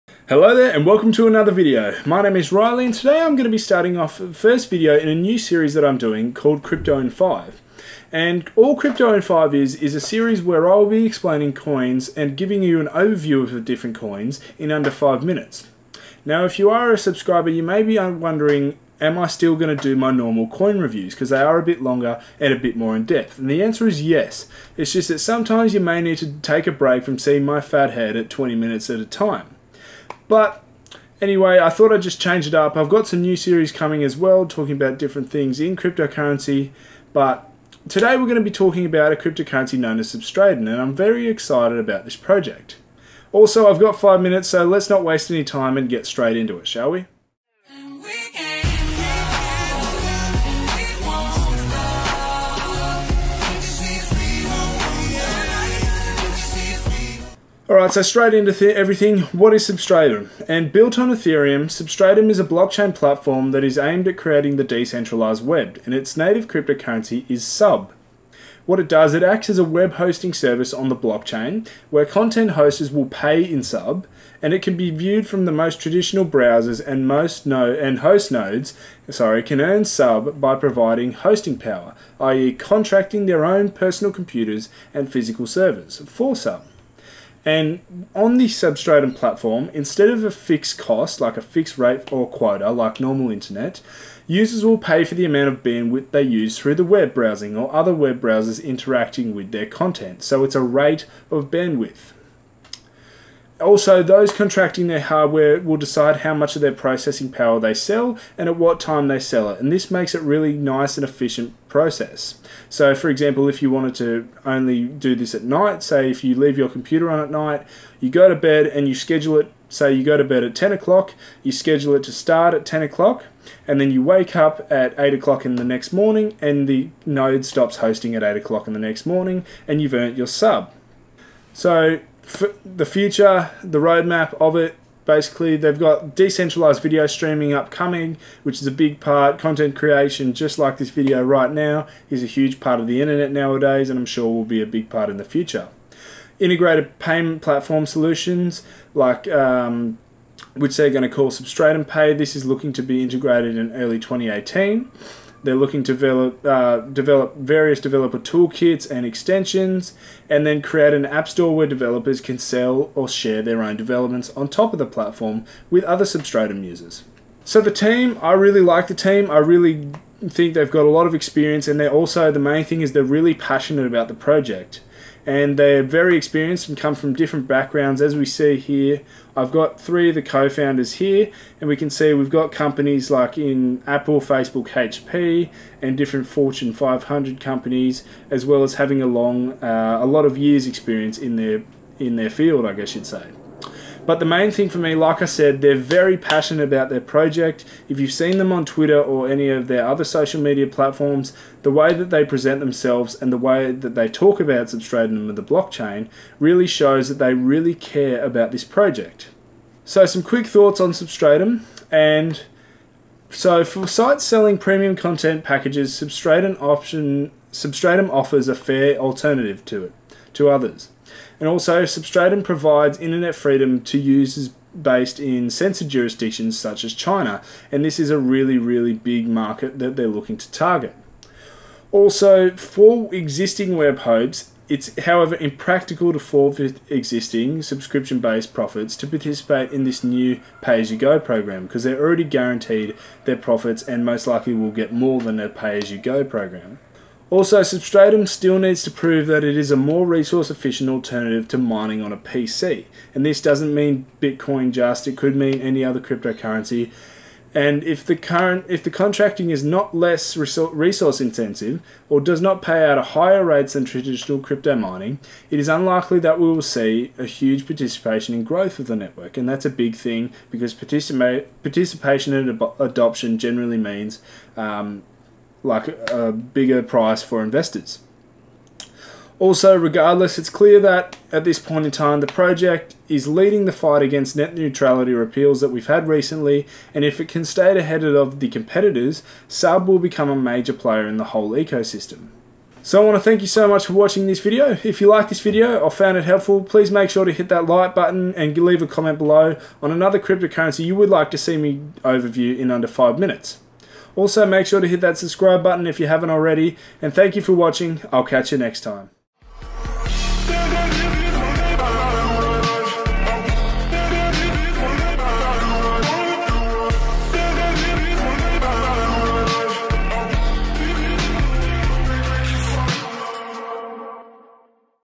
SUBSTRATUM IN 5 | WHAT IS SUBSTRATUM? | SUBSTRATUM OVERVIEW
This video is the first of my new series called crypto in 5. For this episode, i will be giving you a brief outline on the cryptocurrency substratum. Sub is looking to bring the decentralised web on the blockchain and with their great team i am very likened to this project.

BTC: 1Perj6GD1BPQpeJYo1NHXcnhRBh5fnzCGu
BCH: 17RvdJgMN4MMbLobCsYZM5rDLaC9shYrEz
ETH: 0xf402dDF581C1eA0fa48cD14b2Da76f1857034C0c
DASH: XcqDG3WfZye8k2BNVaZu2rLwEvyYb9d6hp
LTC: LceY4PyGGbsipM56PMrKQBhYr3aWpkfGxt
OMG: 0xf402dDF581C1eA0fa48cD14b2Da76f1857034C0c